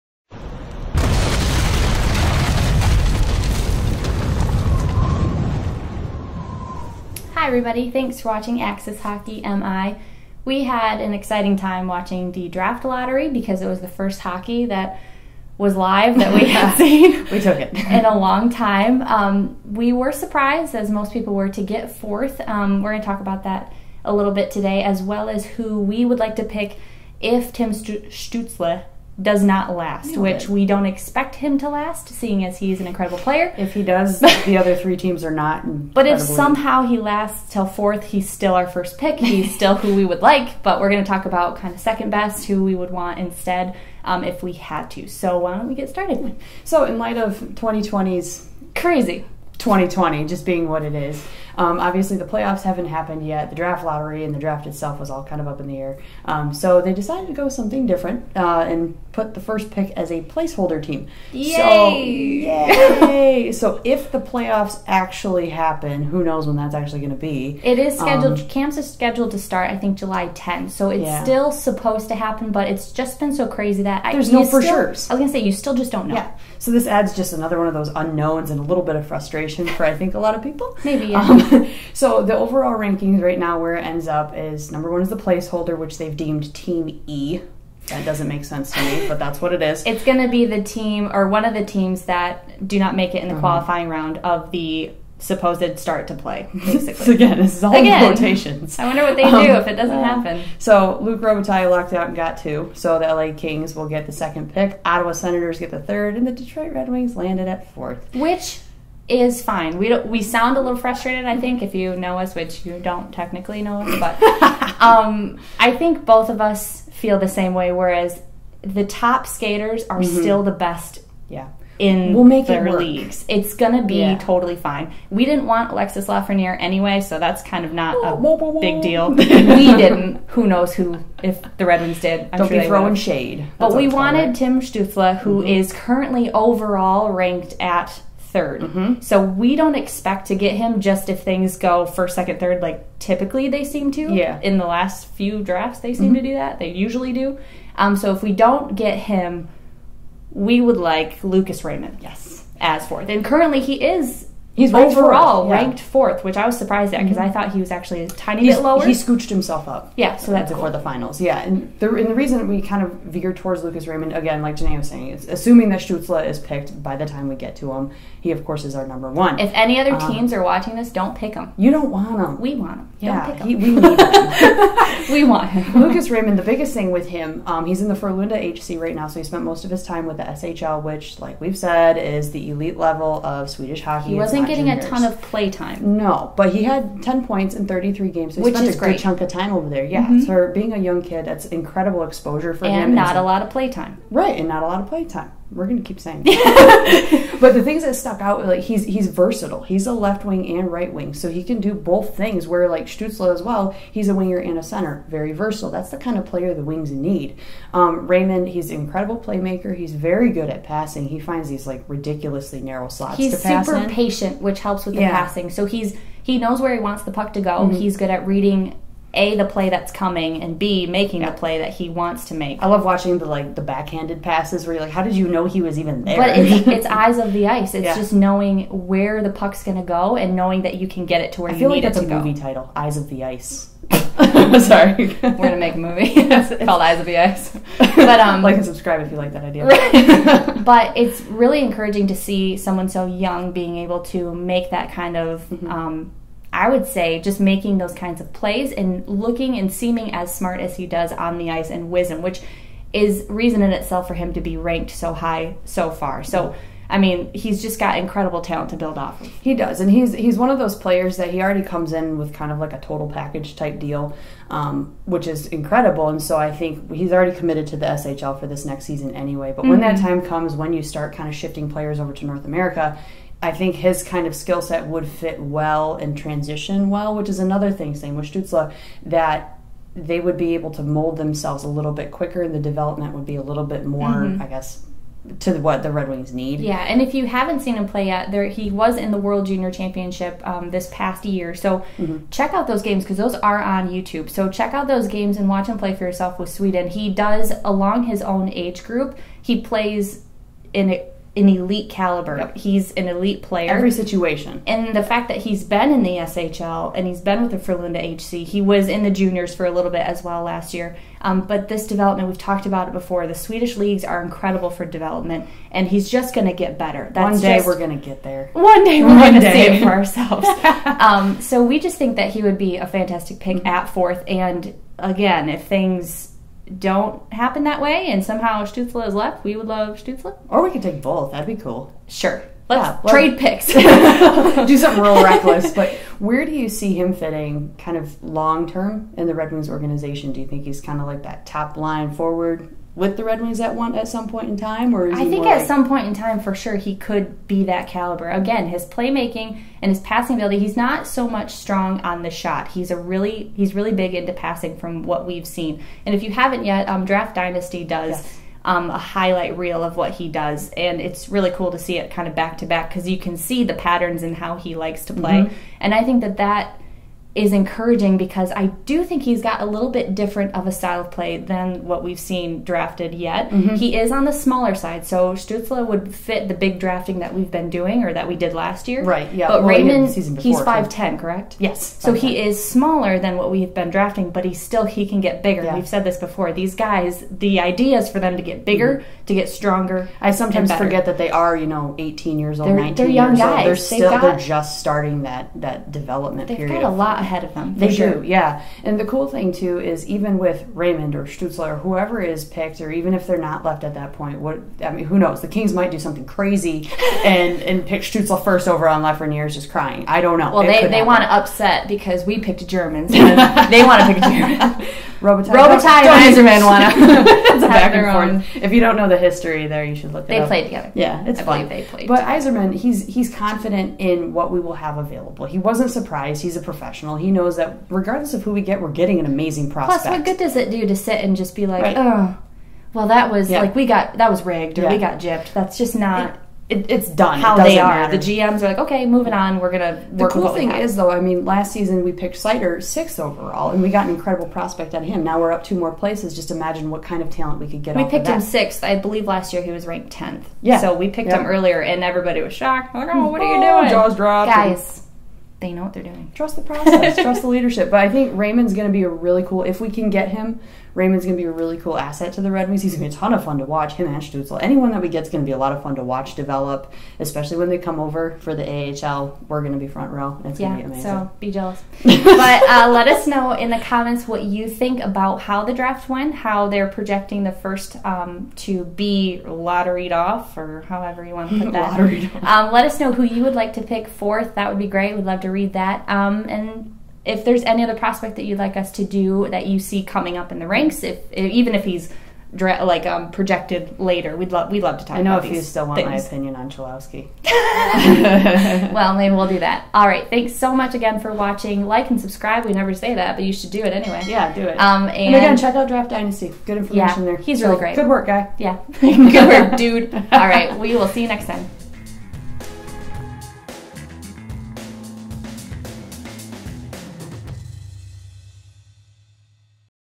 Dear Red Wings - If Not Stutzle, then Raymond?
The NHL Draft Lottery results are in and there are few who are as confused as Detroit Red Wings fans. After the worst season in recent memory, the Wings had the highest odds of picking first - until the NHL announced that first would be awarded to a placeholder team. A team designated "E" and that would be decided upon should the playoffs proceed as planned.
With that, we came to the conclusion that its unlikely Tim Stutzle would be available at fourth overall, the slot awarded to Detroit. So we think Lucas Raymond would be a nice fit for a reasonable second choice.

Who would you pick fourth? Shout out to @draftdynasty for the epic highlights we got to watch to prep for the video. You do good work, friend!